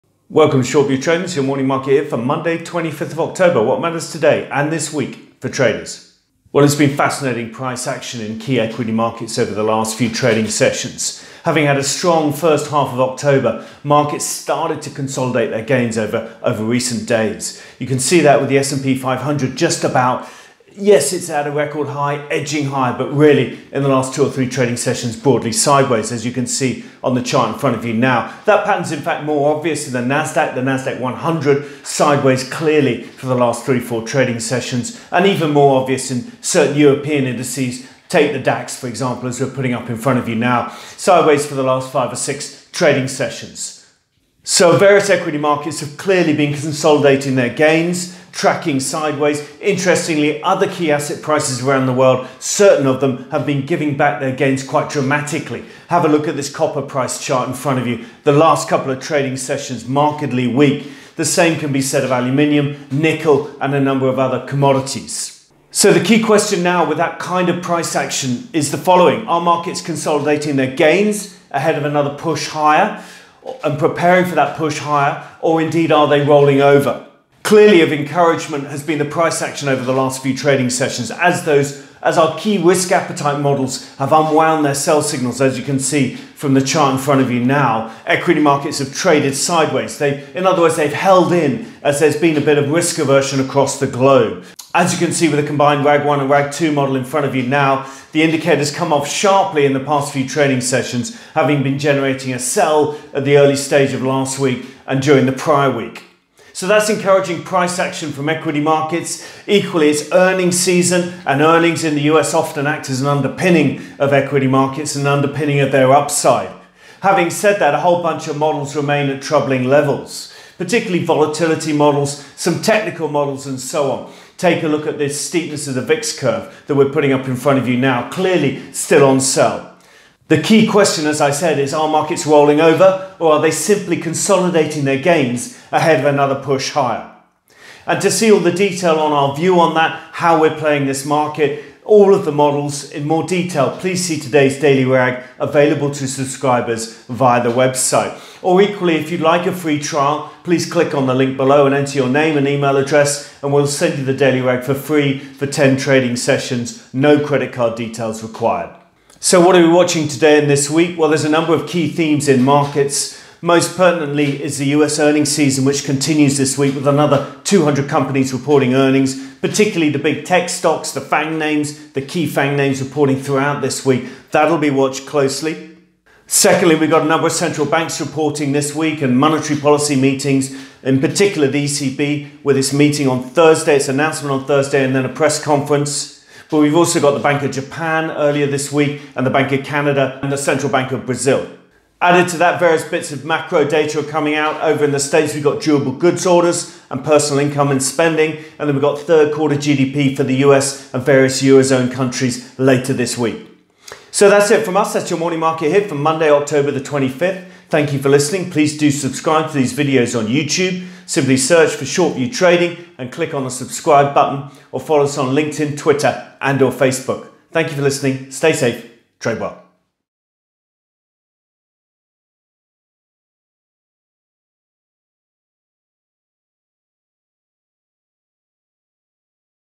Consolidating gains? Or something more sinister?
What matters this week for traders:

• US earnings season continues (200 companies reporting this week, including Facebook, Microsoft, Apple, Amazon and Google)
• Central banks reporting and monetary policy meetings (ECB)
• US macro data e.g. durable goods orders, personal incoming and spending
• Third quarter GDP for US and various Eurozone countries